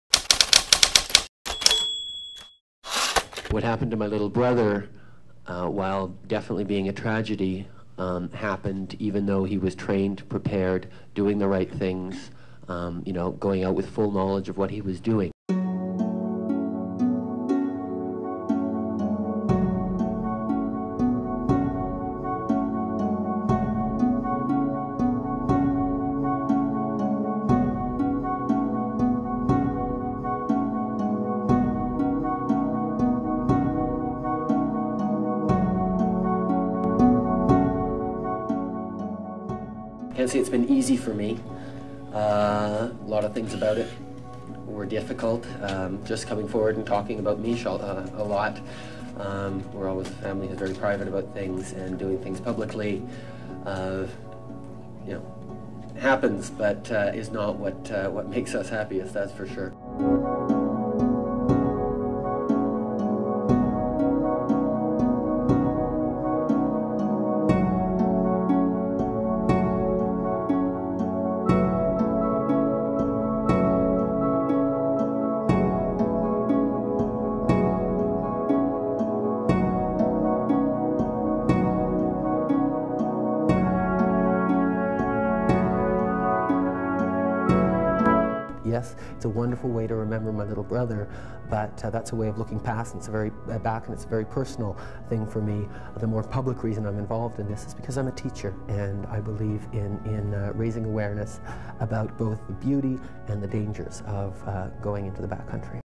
Feb. 3, 2001: Trudeau helps build cabin to reduce tragedies
Feb. 3, 2001: Justin Trudeau launches a campaign to get the Kokanee Glacier cabin built after his brother Michel died in an avalanche.